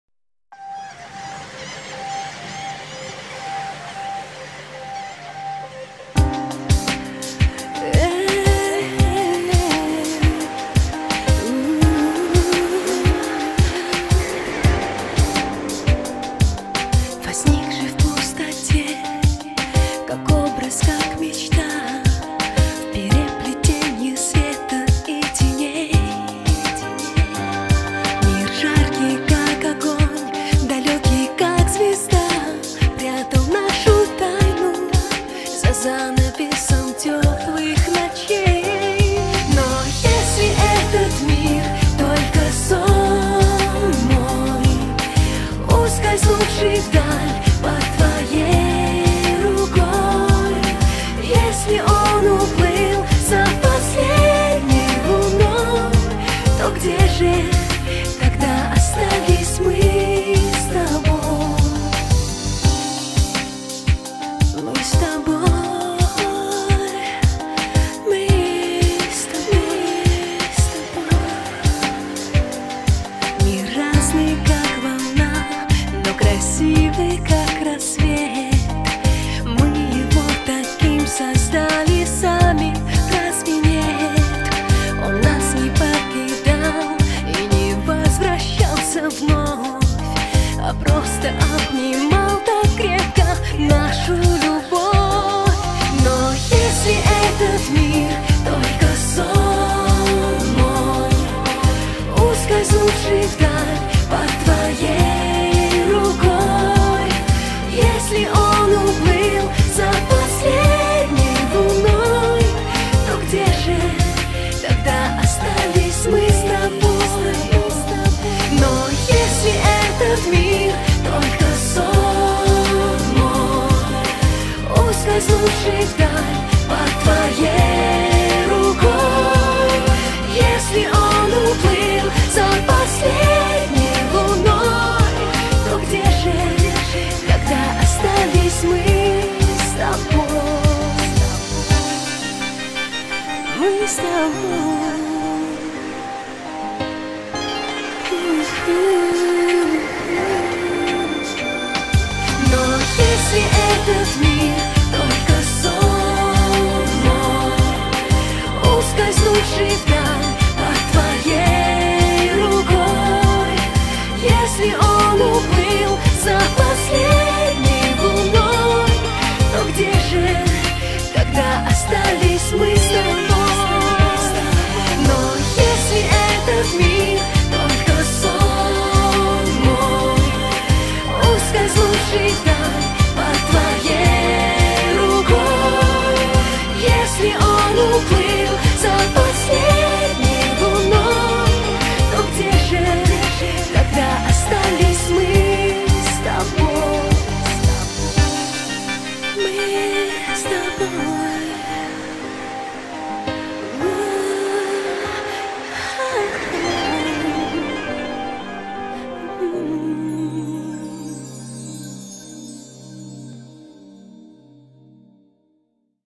Nastasia Music Video
Music video of Russian pop star Nastasia. Produced and directed by Vesko Urukov in Miami Beach, Florida